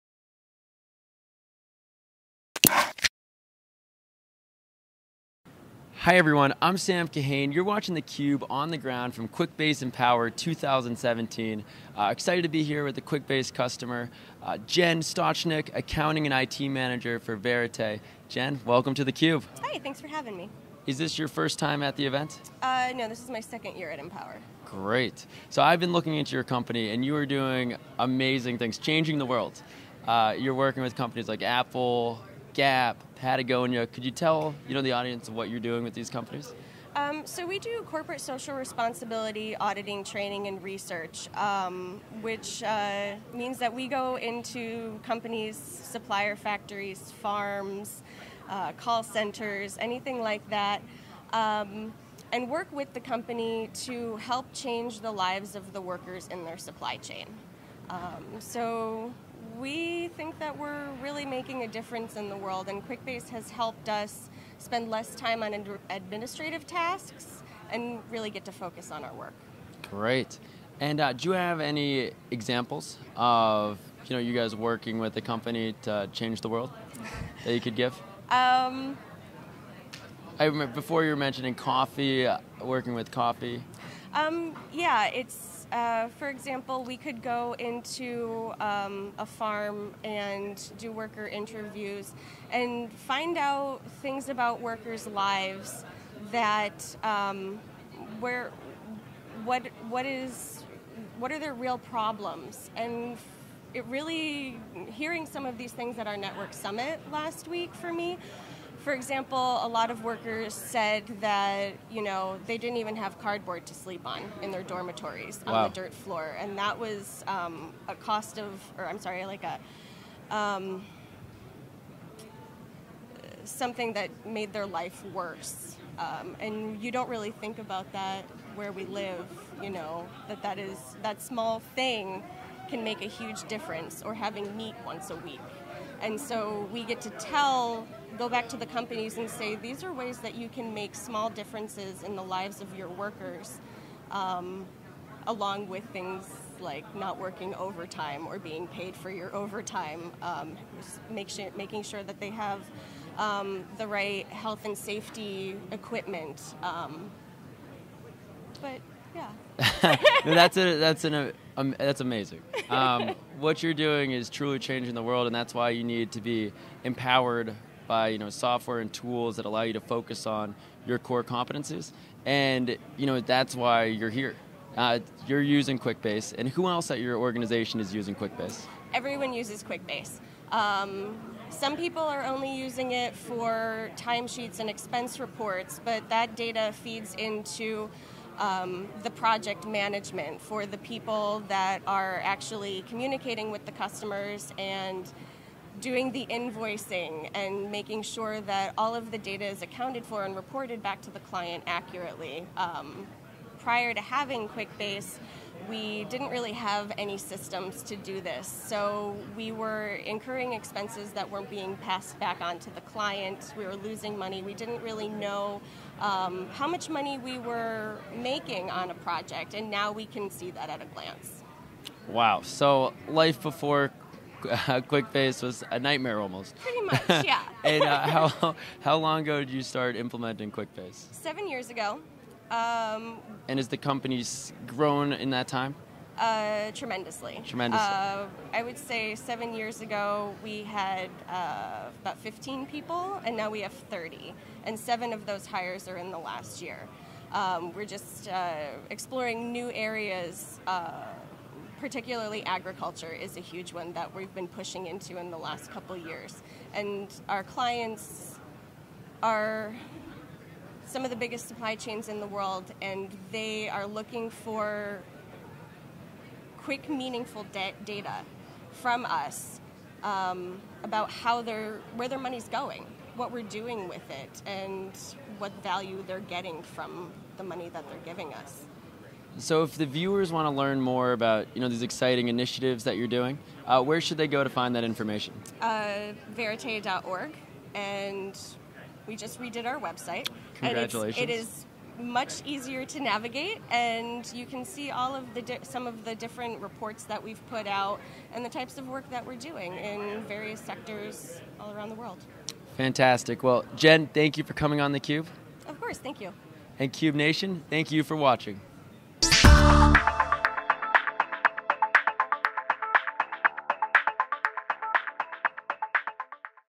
Jenn Stachnik, Verite | Quick Base Empower
Jenn Stachnik, accounting and IT manager at Verite joins Sam Kahane live on the Cube from Quick Base Empower 2017